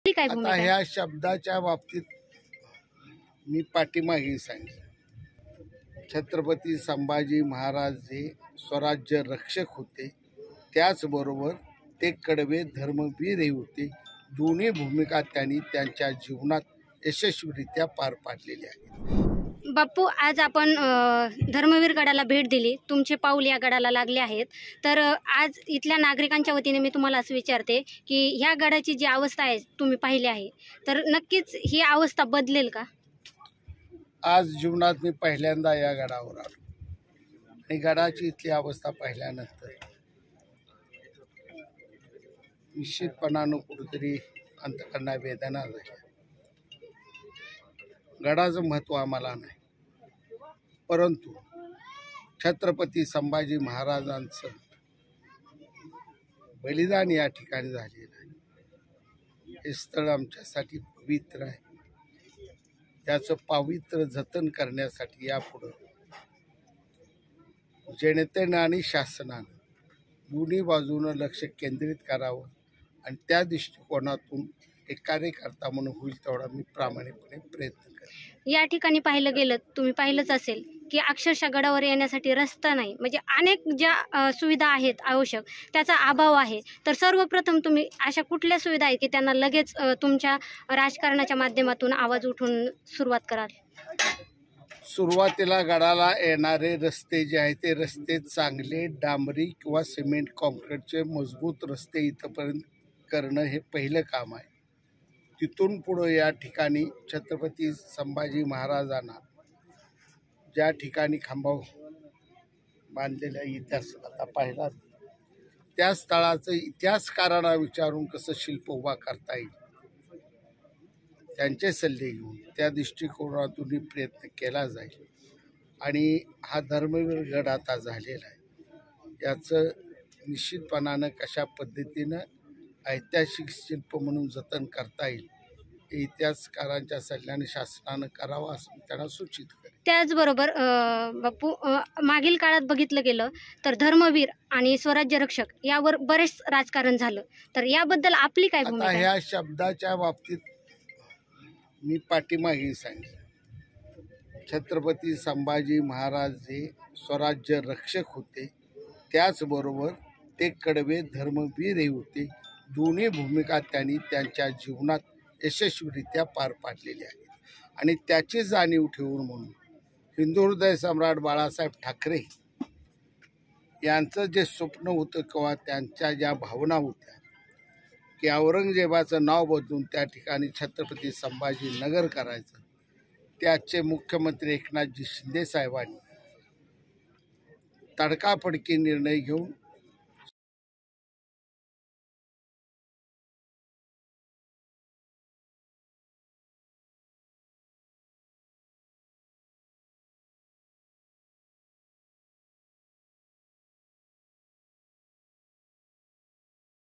MLA Shahajibapu Patil : छत्रपती संभाजी महाराज स्वराज्यरक्षकच.. | LetsUpp Marathi
छत्रपती संभाजी महाराज हे स्वराज्यरक्षक होतेच, त्यासोबतच कडवे धर्मवीरही होते, अशी प्रतिक्रिया सांगोल्याचे आमदार शहाजीबापू पाटील यांनी दिली आहे. आमदार पाटील आज अहमदनगरमधील श्रीगोंदा तालुक्यातील धर्मवीर गडावर आयोजित कार्यक्रमाला आले होते. यावेळी त्यांनी प्रसारमाध्यमांशी संवाद साधला.